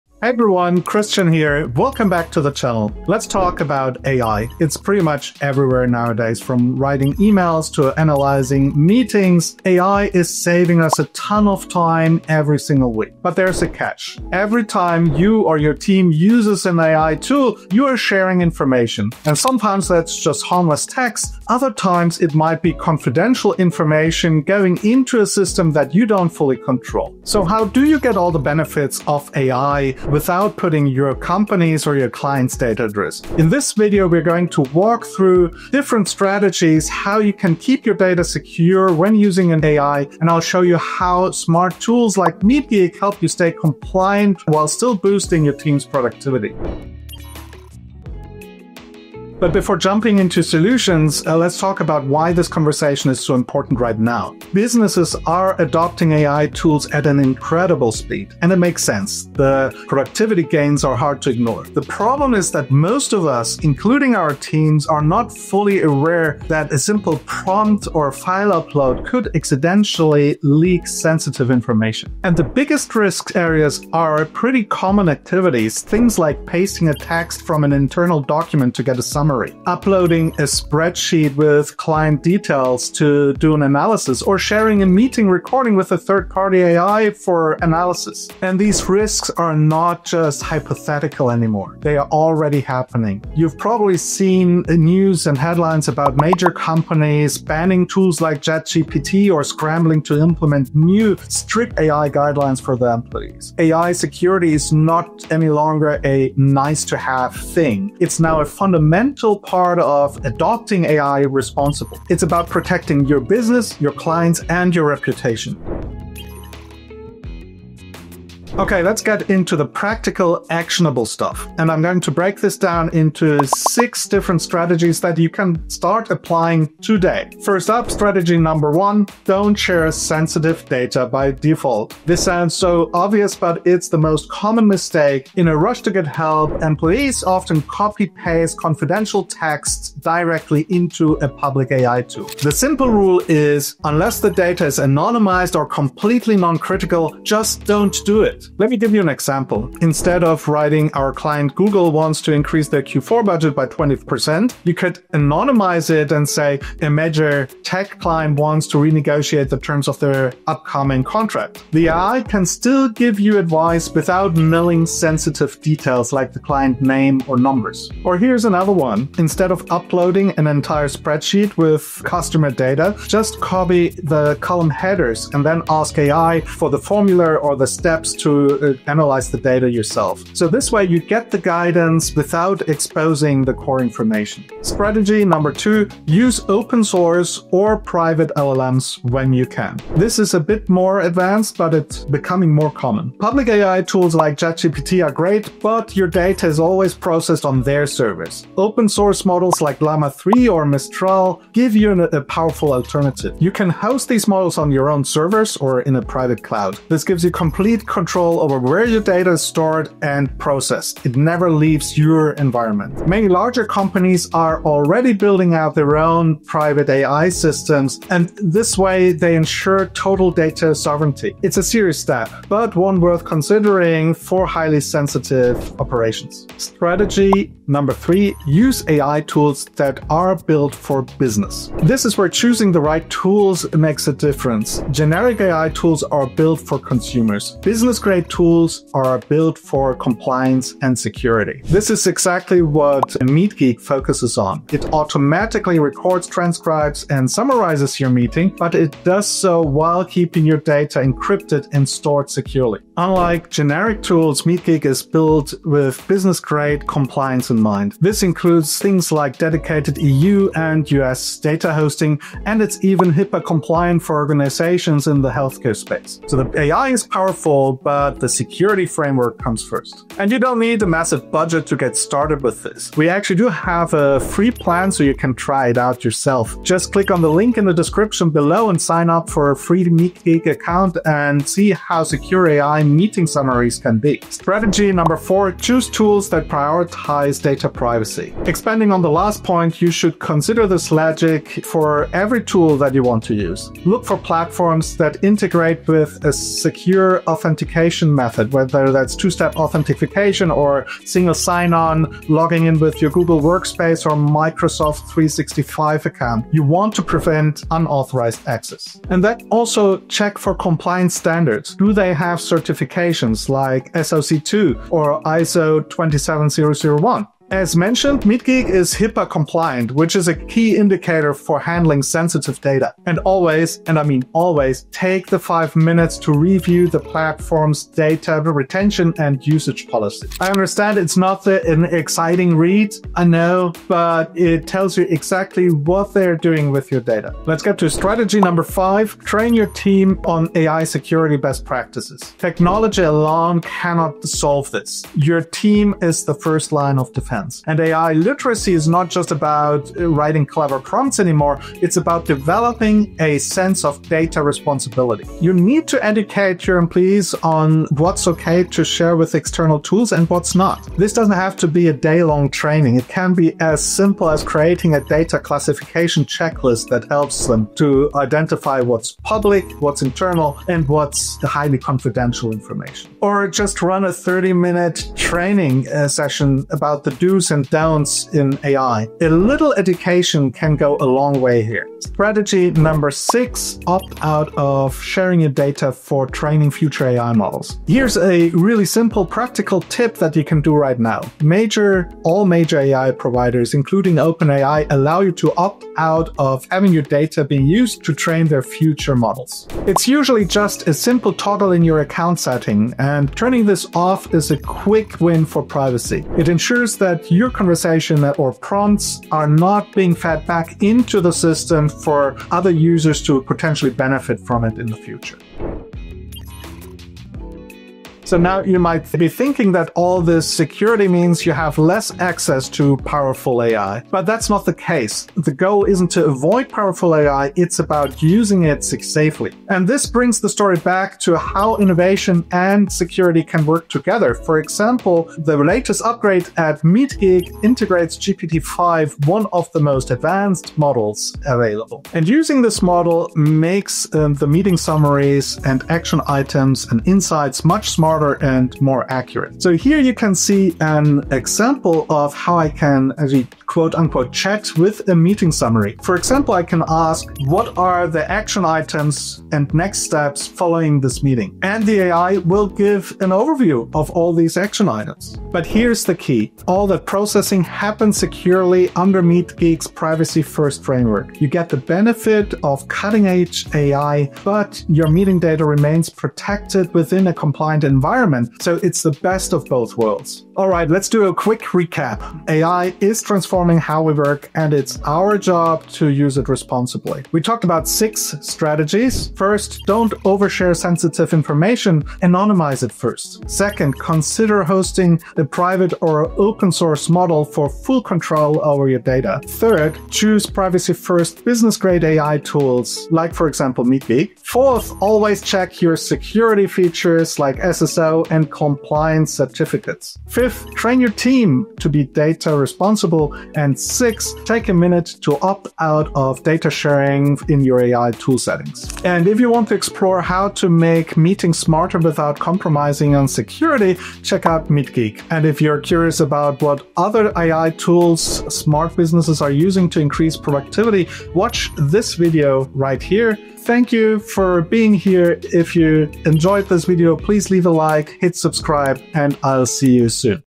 How to Keep Company Data Secure When Using AI (Top Strategies)
AI tools are everywhere — from writing emails to summarizing meetings — but every prompt or upload can expose sensitive company data. This leads to data security risks. In this video, we break down six proven strategies to help you keep your company’s data safe while still enjoying the productivity benefits of AI.

In this video, you’ll discover:

✅ How to protect confidential information when using AI tools
✅ The difference between public, open-source, and private LLMs
✅ Why business-grade AI platforms like MeetGeek are essential for compliance
✅ What security standards (like SOC 2, ISO 27001, and HIPAA) actually mean
✅ How to train your team on responsible AI use
✅ And a simple privacy setting you can toggle today to stop your data from being used to train future models

AI security isn’t optional anymore — it’s the foundation of responsible innovation. Whether you’re a startup founder, team leader, or IT manager, these practical steps will help you safeguard client data, stay compliant, and build trust across your organization.

Timestamps:

00:00 - Introduction
00:48 - Why AI Security Really Matters
01:56 - Top 6 Strategies to Keep Your Data Secured
06:56 - Secure AI In Action: MeetGeek + GPT-5
08:00 - Quick Recap